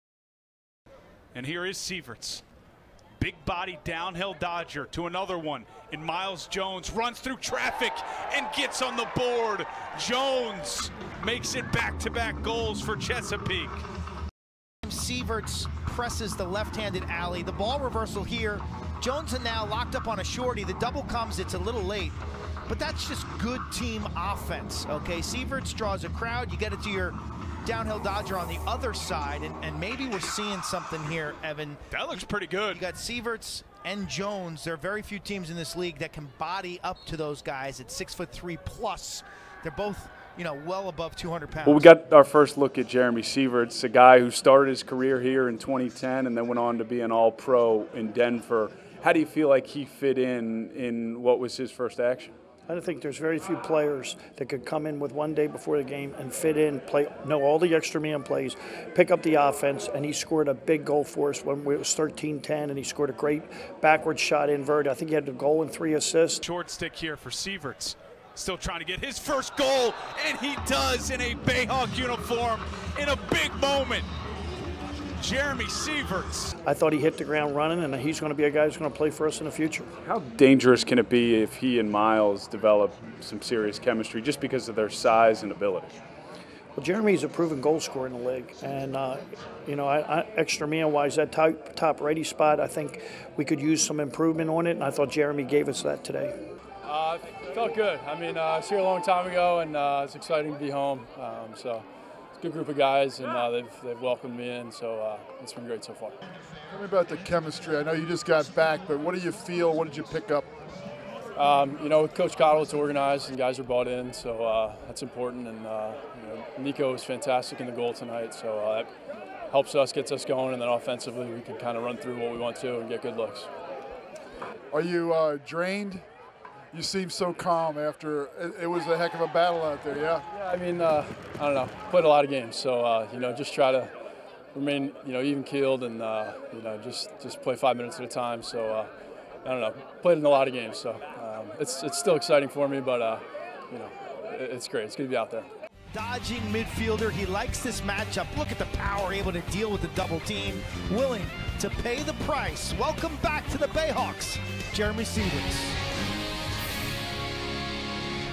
STORY /// Bayhawks Welcome Back Jeremy Sieverts
In his first game back with the Chesapeake Bayhawks since 2010, Jeremy Sieverts makes his presence known on the field. Watch as he hits the ground running for the Bayhawks!